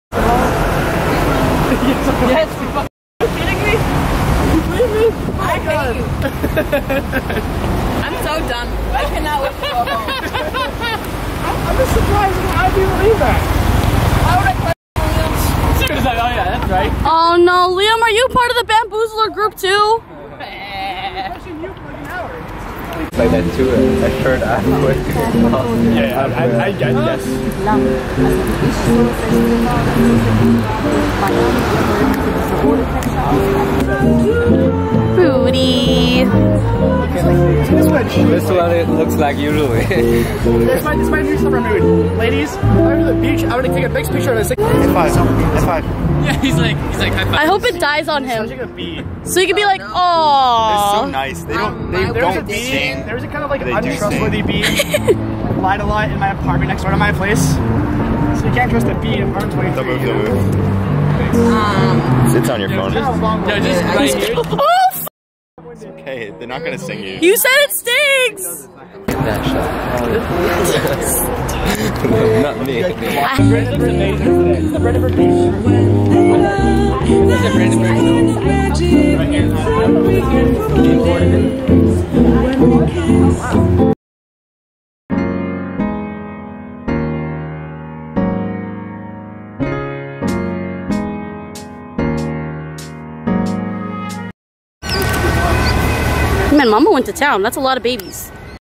BEE ALMOST STUNG ME IN HEIDELBERG | Heidelberg Vlog
Chris said the bees were nice but I don't trust him or the bees in Heidelberg.

♡ katelynn cassidee rae

ABOUT KATELYNN CASSIDEE RAE:
Hi! I take you on my adventures and chat about my life in hopes of inspiring you to live your best life! ☺